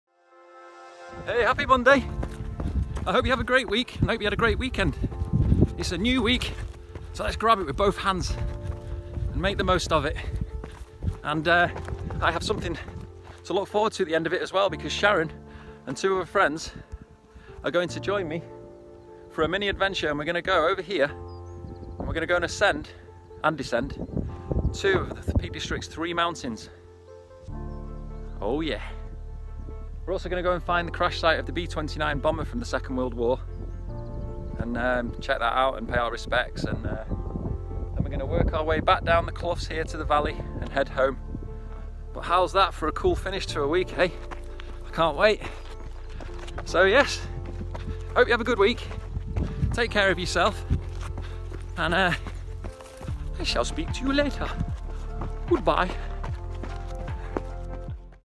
60 Second Peak District Run (#3)
Just saying "Hi!" whilst out running in my hills. 😁